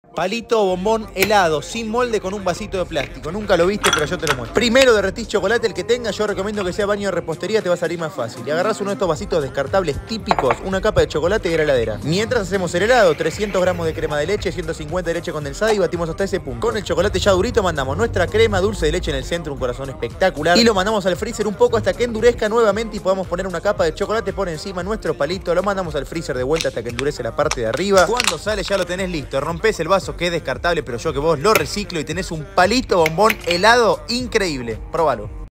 PALITO BOMBÓN HELADO: sin molde, sin máquina y súper fácil 🍦
PALITO BOMBÓN HELADO: sin molde y sin máquina, pero con un resultado espectacular 💣
Este helado es un clásico que le gusta a todo el mundo y esta versión no puede ser mas fácil! No necesitas ningún molde especial, ninguna máquina rara (si no tenes batidora eléctrica, lo haces a mano sin problema) ni mucho tiempo de preparación 🙌🏻
𝕀𝕟𝕘𝕣𝕖𝕕𝕚𝕖𝕟𝕥𝕖𝕤 (rinde 4 vasitos aprox)
• 300gr Crema de Leche 🥛
•150gr Leche Condensada
•c/n Dulce de Leche (o Nutella)
•c/n Chocolate Amargo (baño de repostería es el más fácil de utilizar) 🍫
Y si les gusto este video… que opinan, hago mas palitos helados?🍦